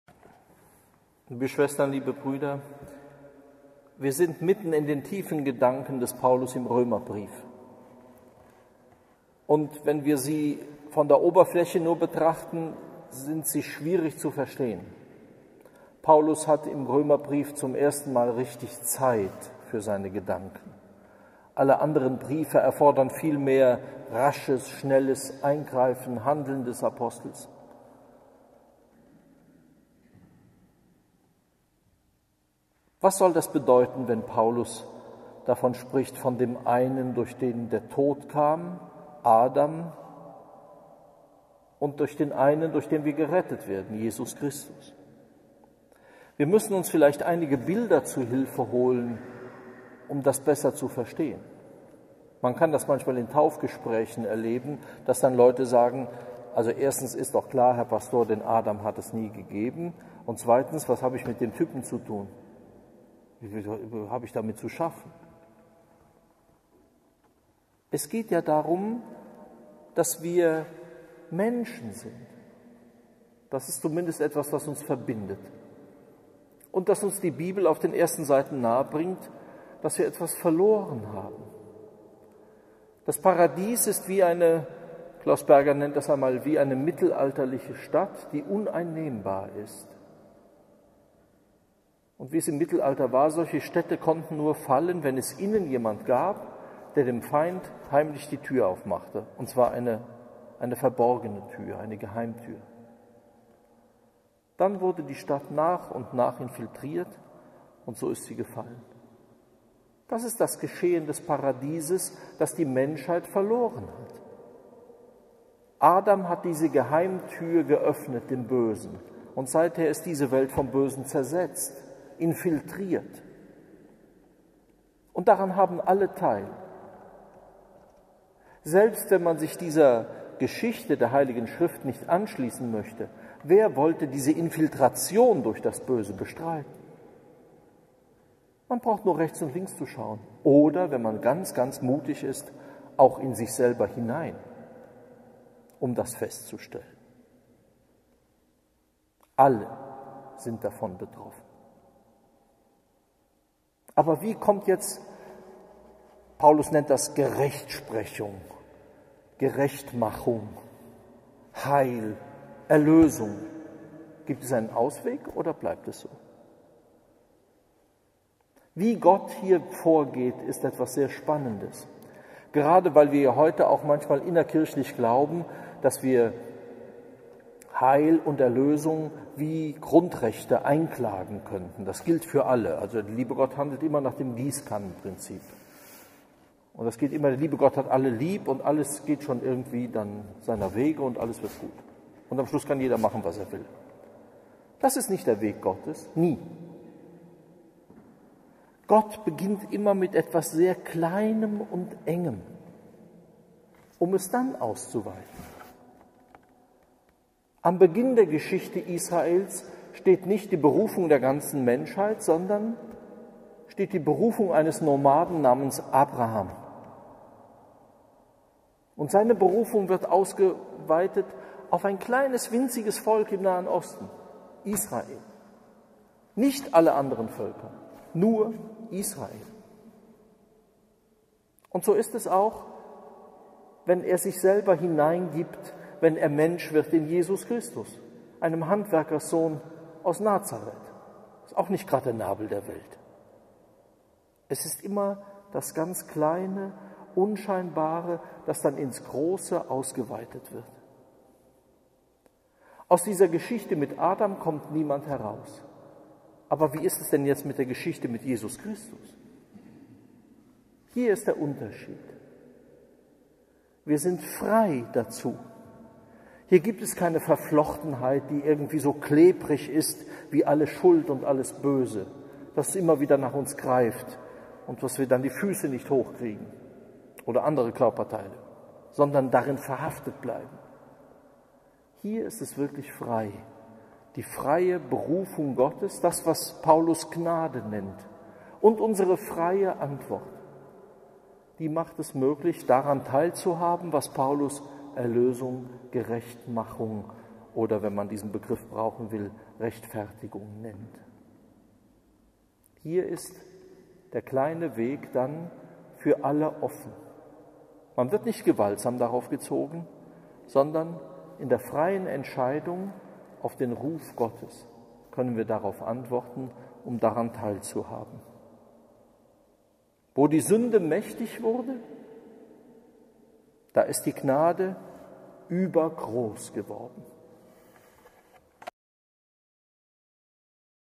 Aus der Geschichte mit Adam kommt keiner raus
Rektor Dr.Johannes J.Kreier im Kloster am Rande der Stadt/ Saarbrücken, Predigt, Di, 28.Wo im Jahreskreis.